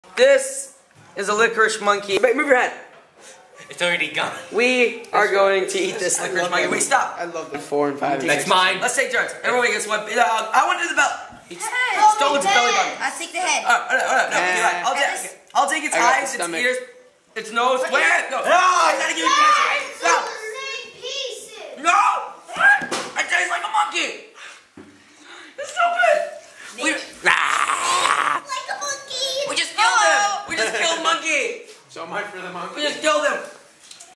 Eating a Liquorice Monkey
Fighting over a liquorice monkey I made. 🙉
Quality entertainment! hahahahaha 🙊 (please don't kill me...).🙈
No animals were hurt in the making of this video (except, of course, the liquorice monkey...).🙈🙈🙈
P.S It didn't actually taste like a monkey.